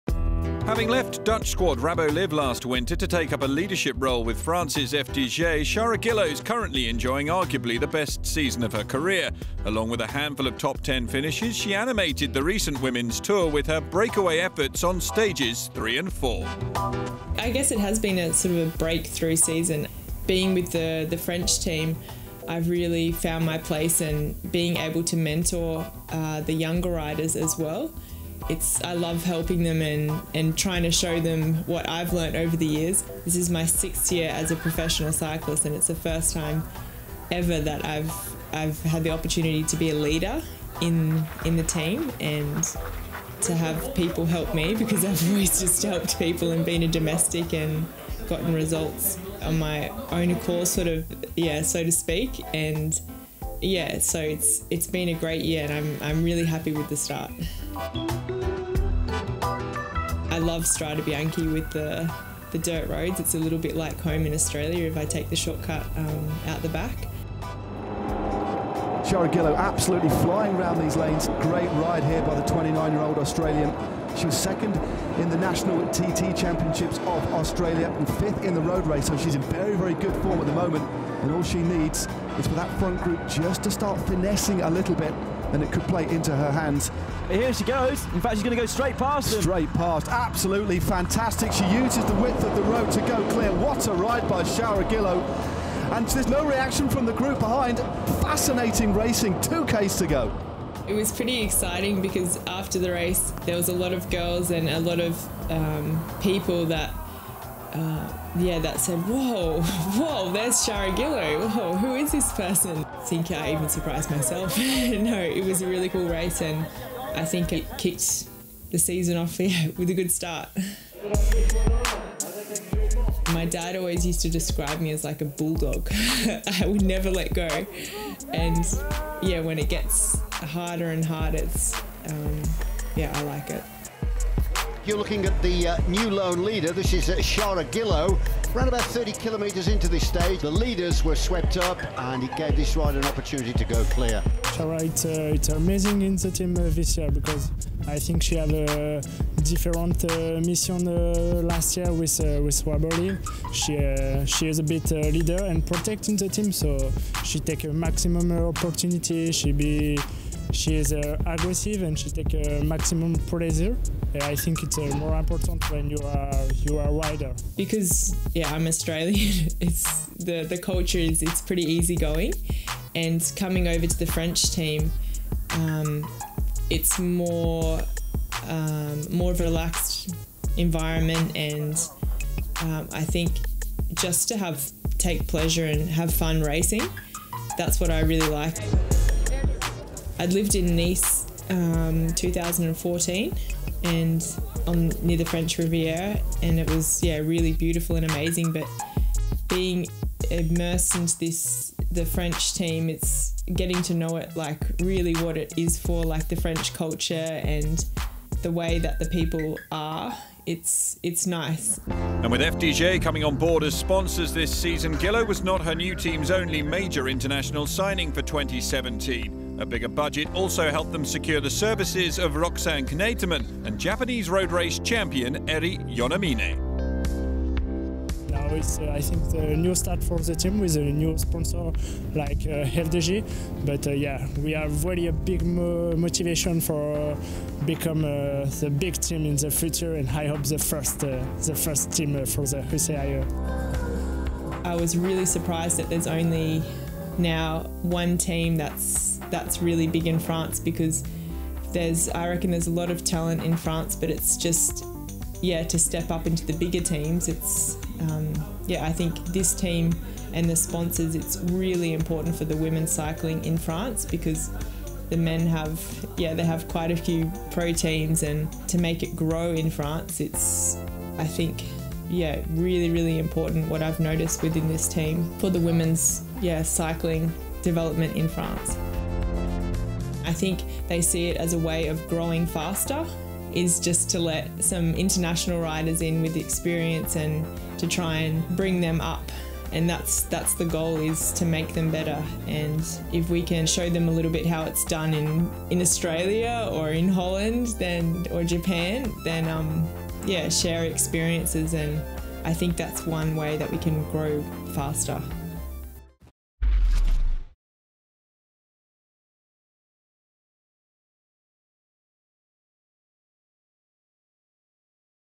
2017 UCI Women's WorldTour - Shara Gillow
In this new episode following the 2017 UCI Women's WorldTour we focused on FDJ's Shara Gillow.